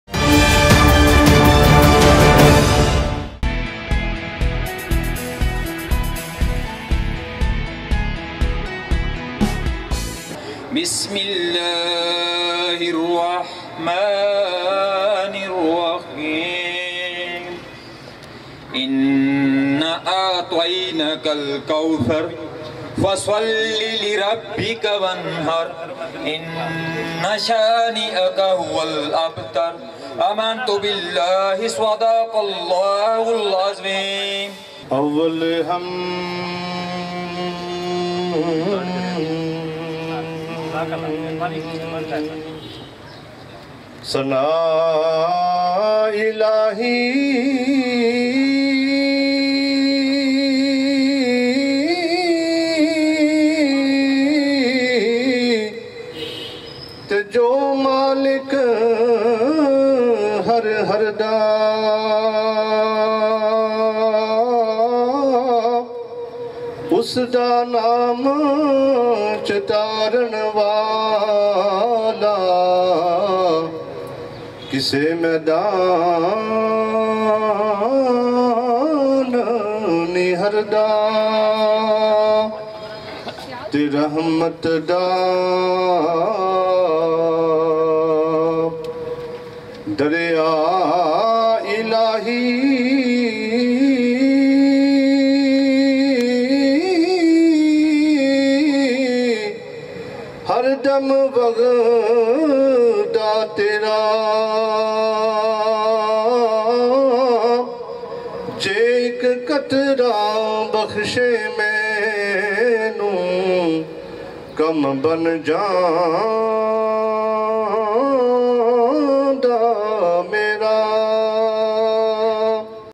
17th Anniversary kalam of mian baksh
سالگرہ کا اغاز تلاوت قران اور میاں بخش کے قلام سے کیا گیا